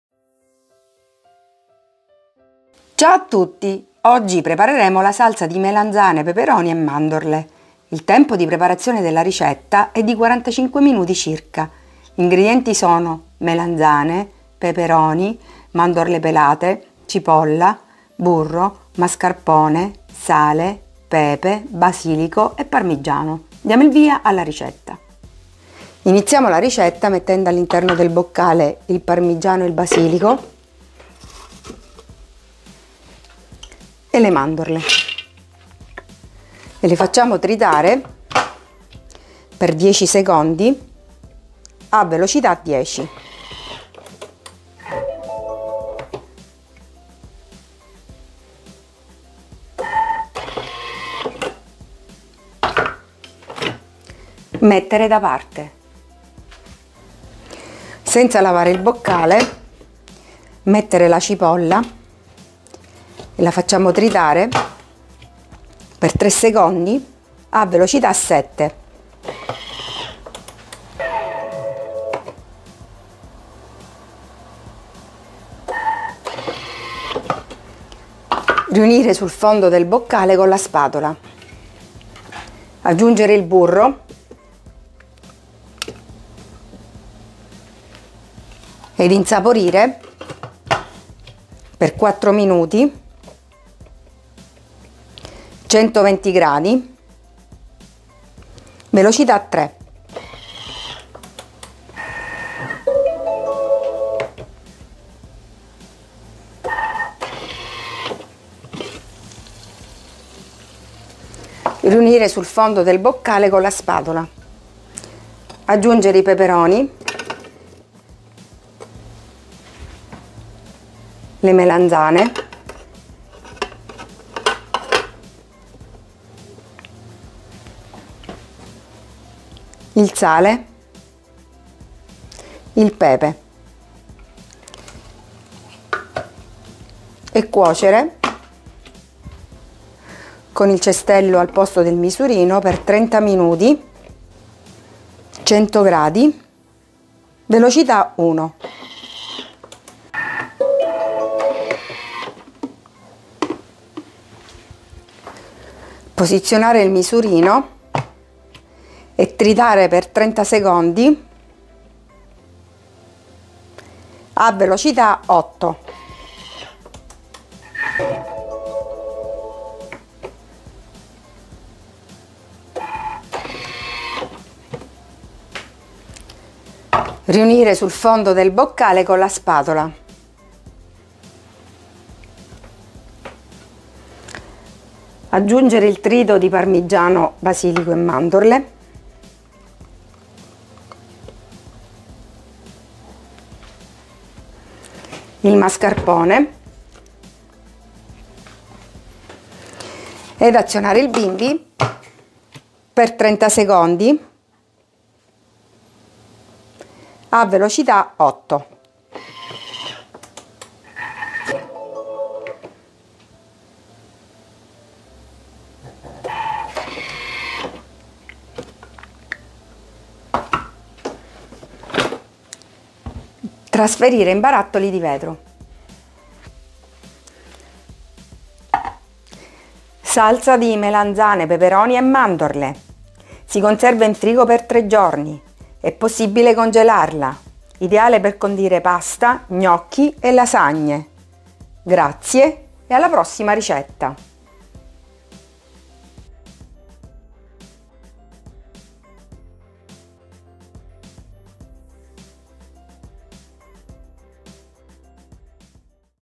Salsa di melanzane peperoni e mandorle per bimby TM6 TM5 TM31
Ingredienti:
400 g di melanzane tagliate a pezzi
400 g fi peperoni tagliati a pezzi
60 g di mandorle pelate
30 g di cipolla
40 g di burro
150 g di mascarpone
sale e pepe q.b.
10 foglie di basilico
50 g di parmigiano

English
ingredients
400 g of eggplants cut into pieces
400 g of peppers cut into pieces
60 g of peeled almonds
30 g of onion
40 g of butter
150 g of mascarpone
salt and pepper to taste
10 basil leaves
50 g of Parmesan

Español
ingredientes
400 g de berenjenas cortadas en trozos
400 g de pimientos cortados en trozos
60 g de almendras peladas
30 g de cebolla
40 g de mantequilla
150 g de mascarpone
sal y pimienta al gusto
10 hojas de albahaca
50 g de parmesano

Deutsch
zutaten
400 g Auberginen in Stücke geschnitten
400 g Paprika in Stücke geschnitten
60 g geschälte Mandeln
30 g Zwiebel
40 g Butter
150 g Mascarpone
Salz und Pfeffer nach Geschmack
10 Basilikumblätter
50 g Parmesan